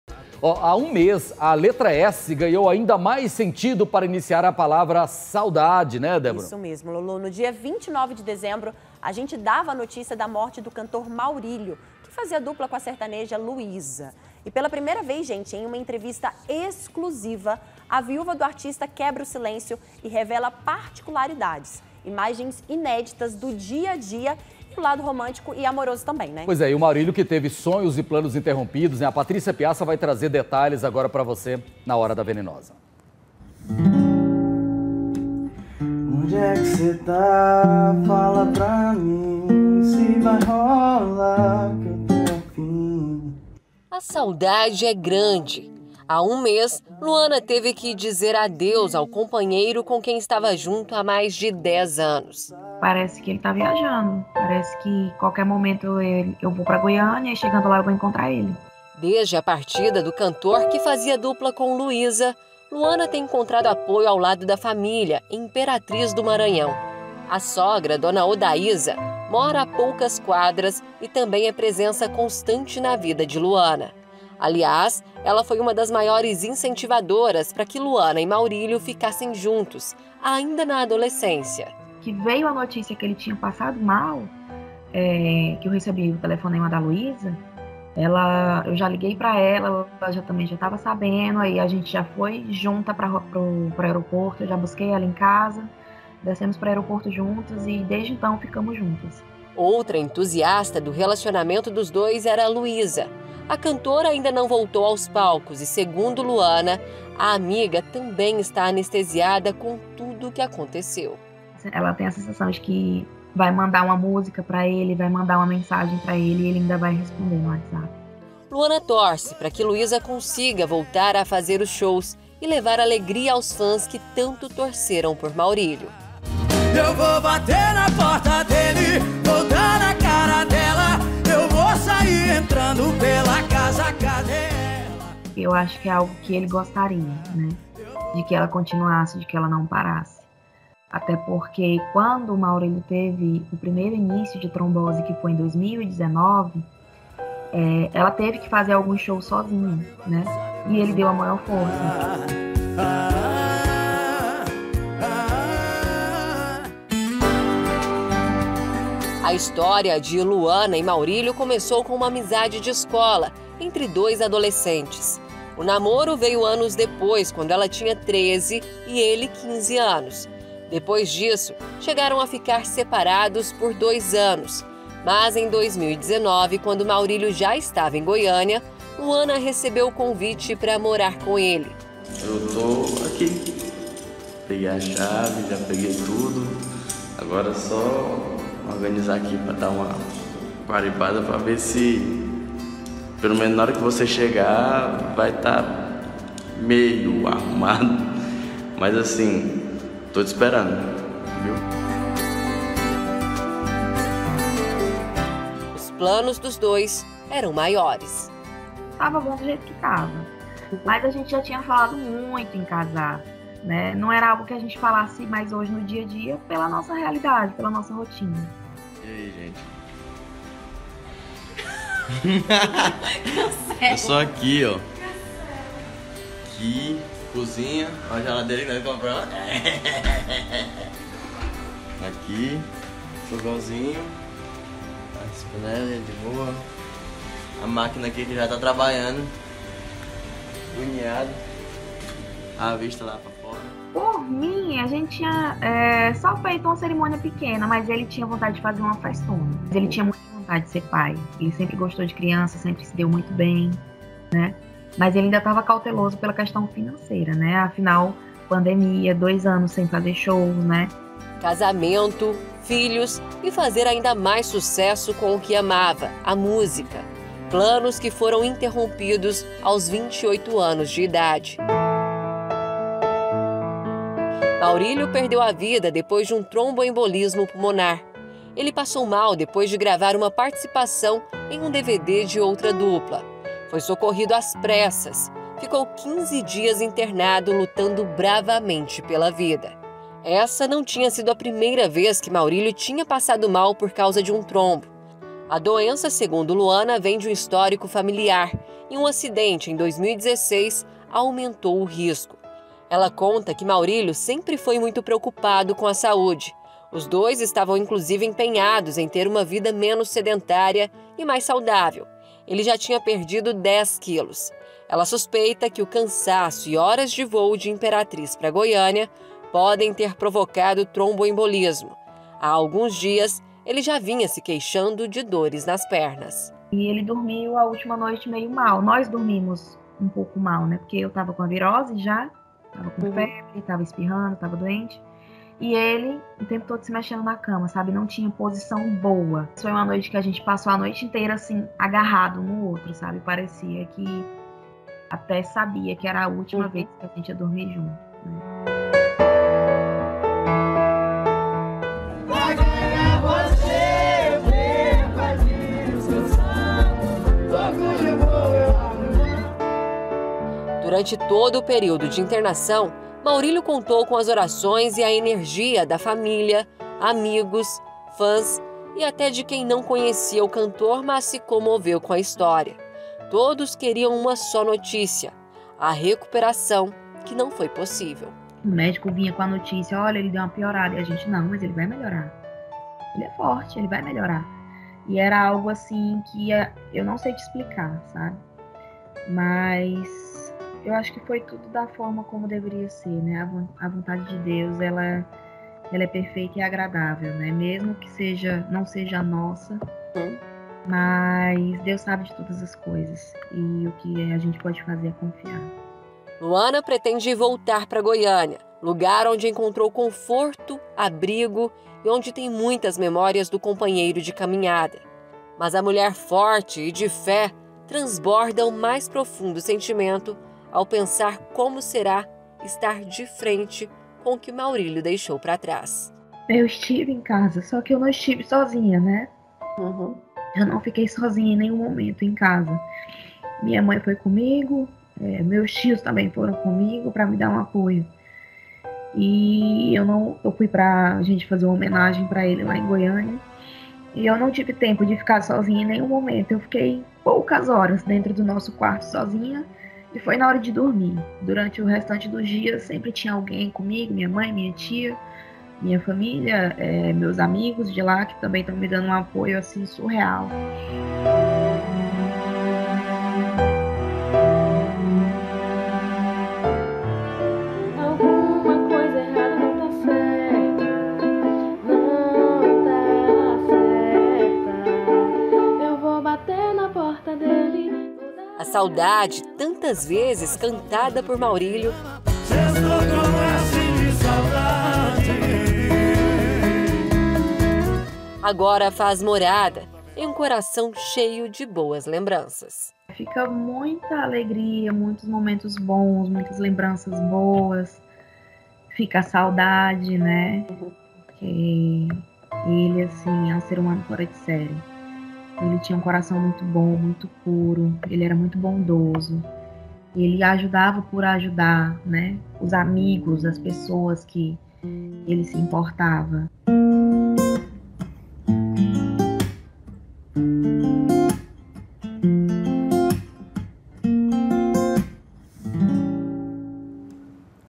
MORTE DO CANTOR MAURÍLIO AINDA É ASSUNTO DOLORIDO E VIÚVA REVELA QUE SUPERAÇÃO ACONTECE AOS POUCOS
HÁ UM MÊS A LETRA "S" GANHOU AINDA MAIS SENTIDO PARA INICIAR A PALAVRA SAUDADE. NO DIA 29 DE DEZEMBRO, A GENTE DAVA A NOTÍCIA DE MORTE DO CANTOR MAURÍLIO QUE FAZIA DUPLA COM A SERTANEJA LUÍZA. PELA PRIMEIRA VEZ, EM UMA ENTREVISTA EXCLUSIVA A VIÚVA DO ARTISTA QUEBRA O SILÊNCIO E REVELA PARTICULARIDADES. IMAGENS INÉDITAS DO DIA A DIA. O LADO ROMÂNTICO E AMOROSO. O MAURÍLIO QUE TEVE SONHOS E PLANOS INTERROMPIDOS.